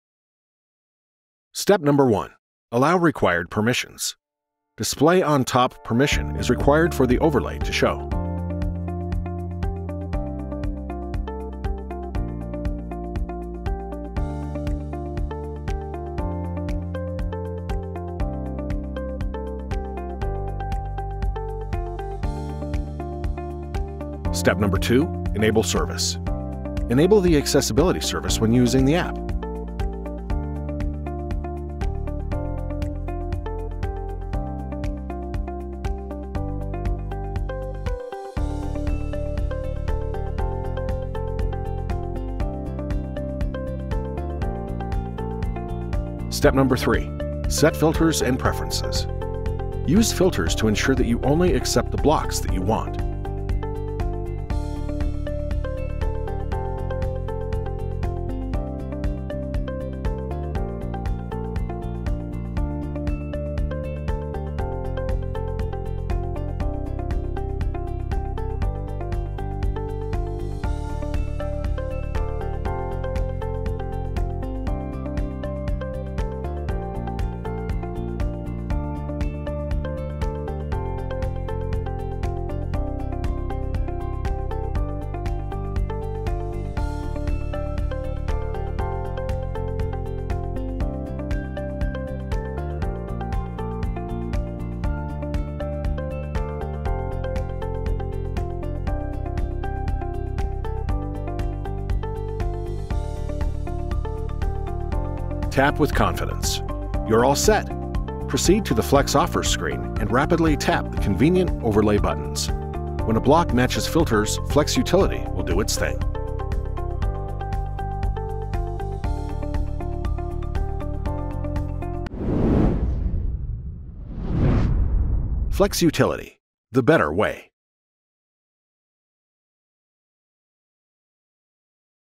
Flex Utility Free - Quick Start Guide (2021)
Quickly get started with Flex Utility Free. Tap with confidence using the powerful filters and convenient overlay buttons.